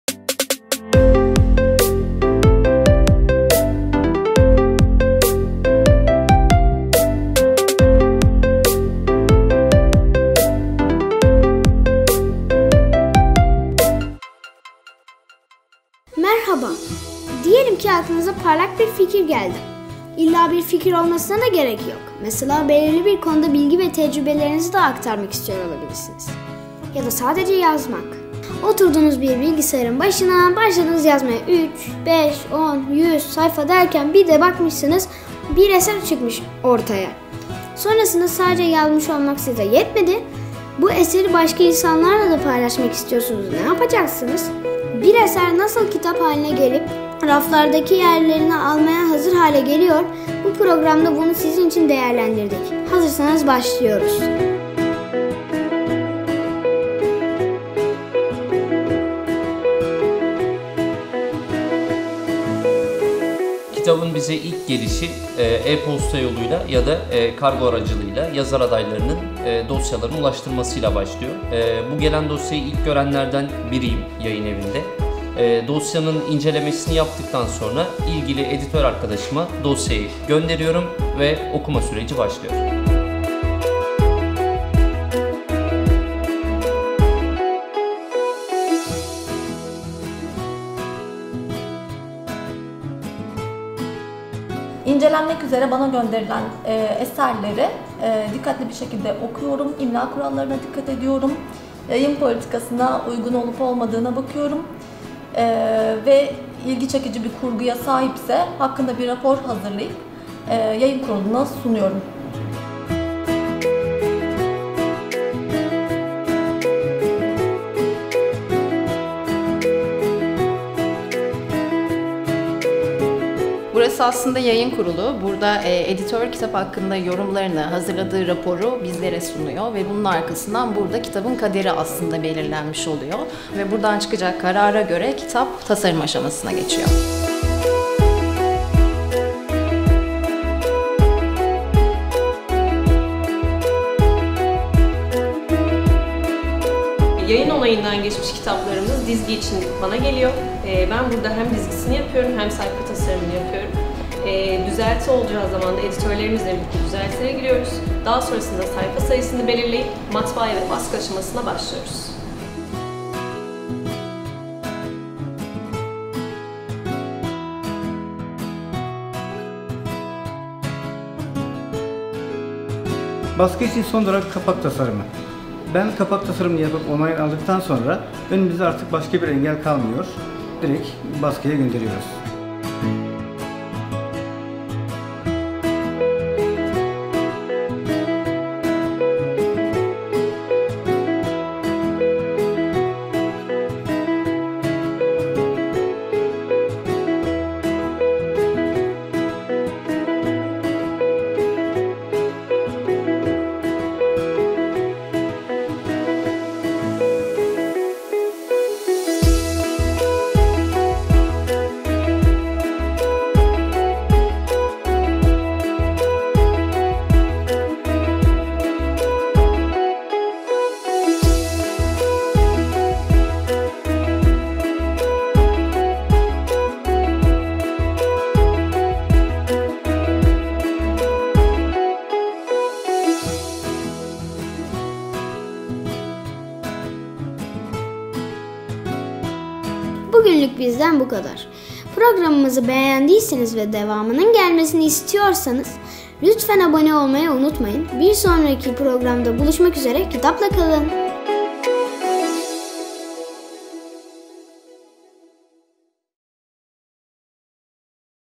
KEREM'İN KİTAPLARI • Kitabın Yolculuğu • StayHome
➤Kitabın Yolculuğu

Diyelim ki aklınıza parlak bir fikir geldi. İlla bir fikir olmasına da gerek yok. Mesela belirli bir konuda bilgi ve tecrübelerinizi de aktarmak istiyor olabilirsiniz. Ya da sadece yazmak...
Oturdunuz bir bilgisayarın başına, başladınız yazmaya... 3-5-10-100 sayfa derken bir de bakmışsınız bir eser ortaya çıkmış. Sonrasında sadece yazmış olmak size yetmedi, bu eseri başka insanlarla da paylaşmak istiyorsunuz, ne yapacaksınız?
Bir eser nasıl bir kitap haline gelip raflardaki yerleri almaya hazır hale geliyor, bu programda bunu sizin için değerlendirdik.
İyi seyirler.

Desteklerinden dolayı İnkılap Yayınevi'ne teşekkür ederiz.

🎧 Müzikler / Musics